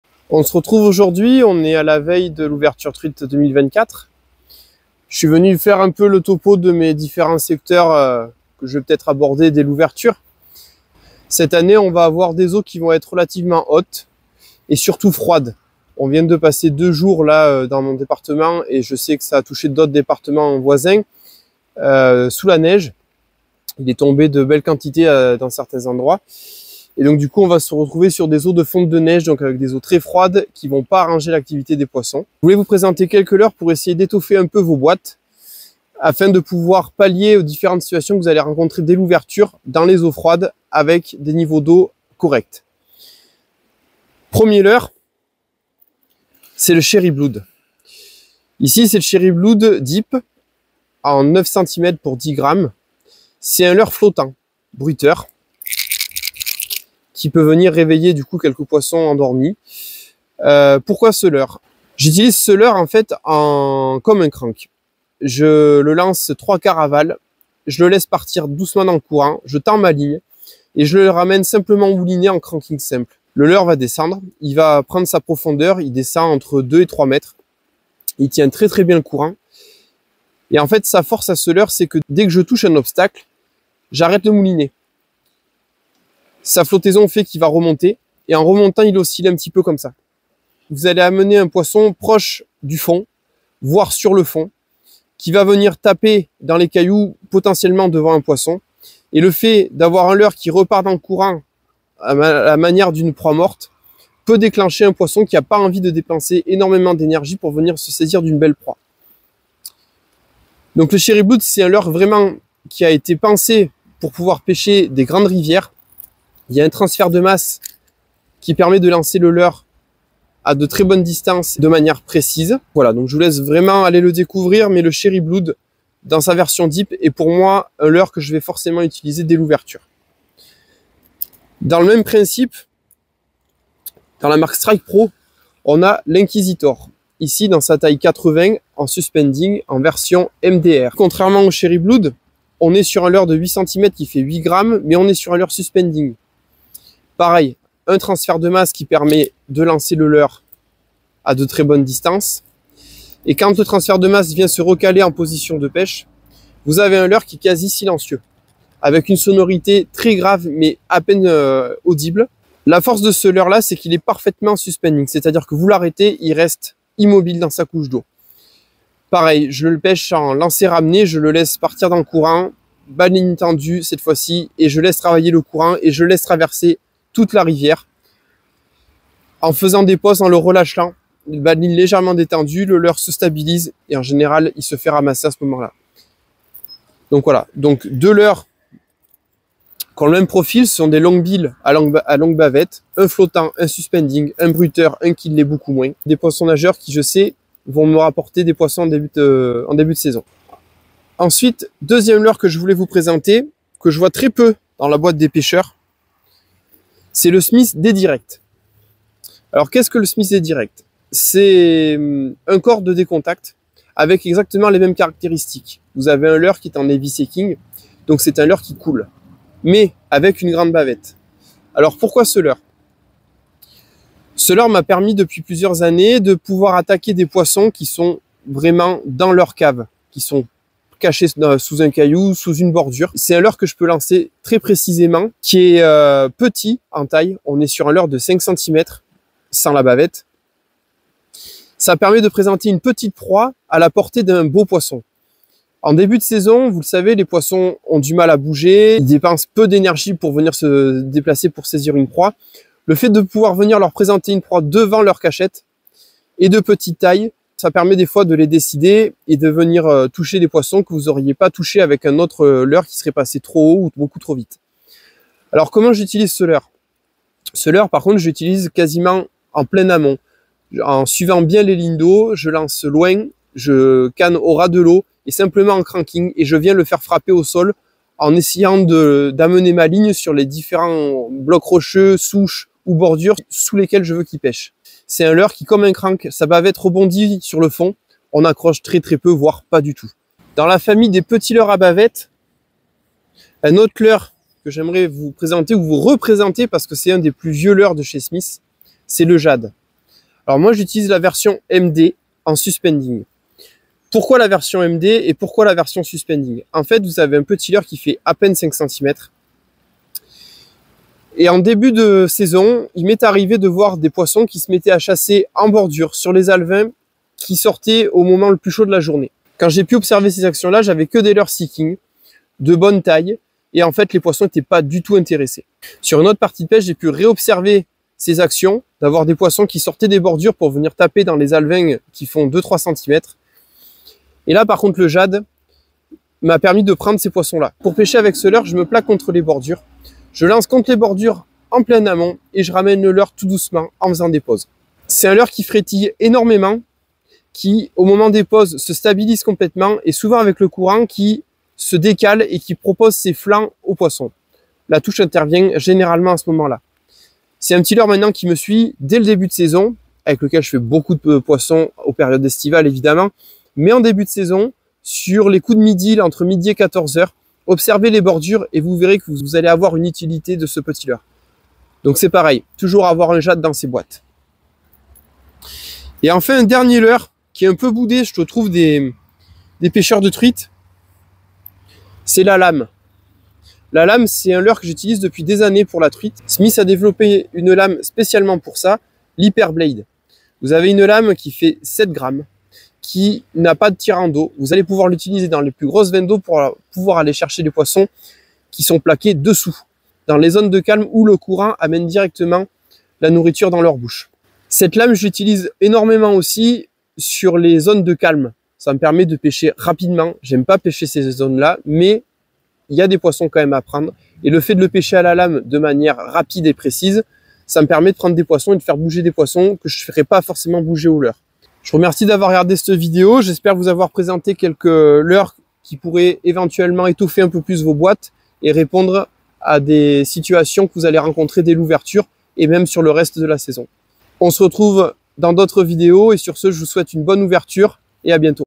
OUVERTURE de la pêche de la TRUITE : la sélection de Julien Sanmiquel
Dans cette vidéo Julien Sanmiquel nous présente sa sélection de leurres en vue de l'ouverture de la pêche de la truite et du début de saison. Ses choix se portent ainsi principalement sur des leurres adaptés aux eaux hautes et au forts débits mais aussi au comportement des truites dans les eaux froides de début de saison.